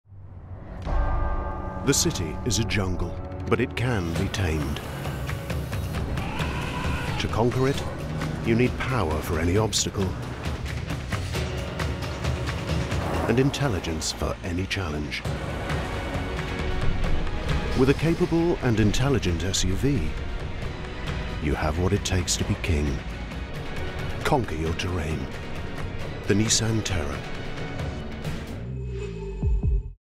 Nissan Terra - Conquer Your Terrain
Conquer all terrain with an intelligent and capable Nissan Terra.